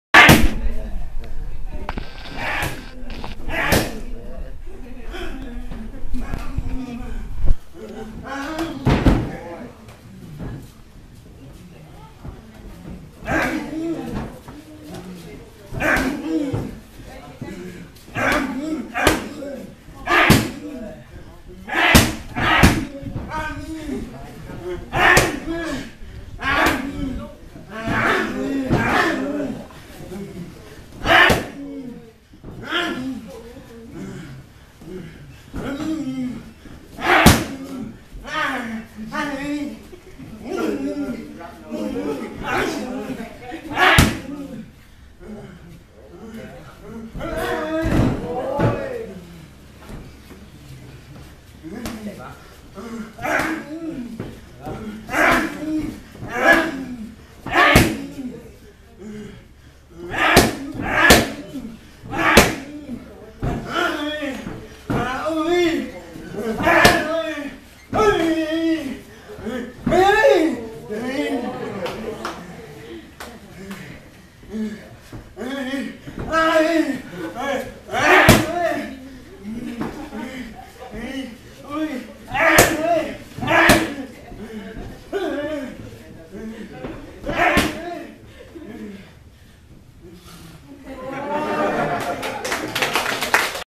Lakhin Wassandasit hitting pads at Koh Yao Noi Muay Thai
Former 3x Thai Muay Thai champion, former OPBF champion Lakhin Wassandasit hitting pads for a tour group visiting Koh Yao Noi Muay Thai

Video recorded July 2012